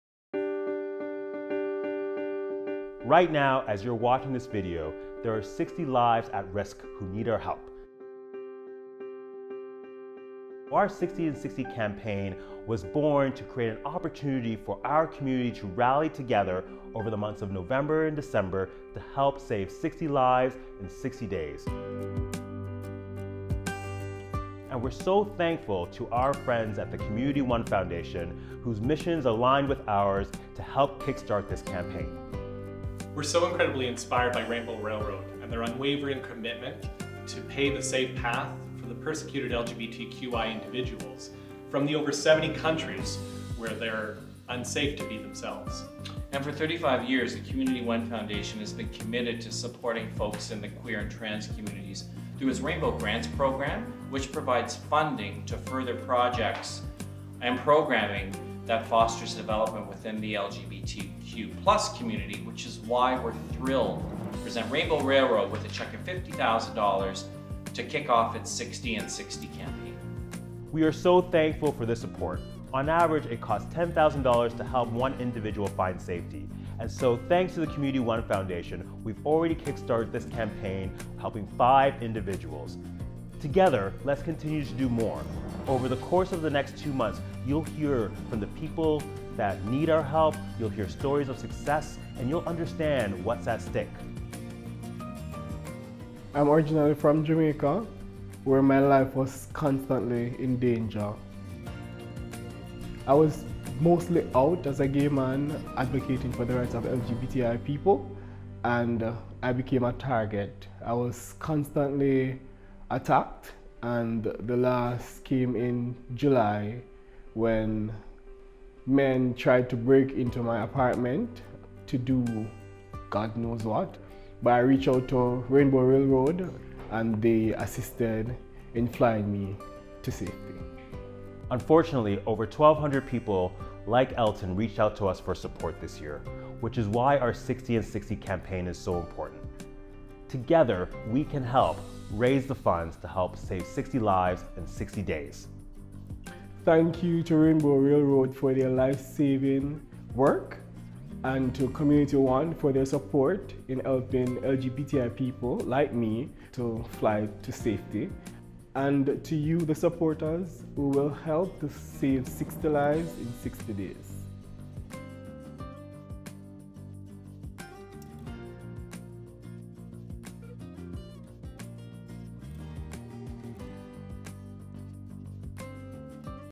Saving 60 LGBTQI lives in 60 days - Rainbow Railroad
60in60 is an opportunity for the international community to come together in the final two months of the year to provide a pathway to safety for 60 LGBTQI people who are ready to travel right now.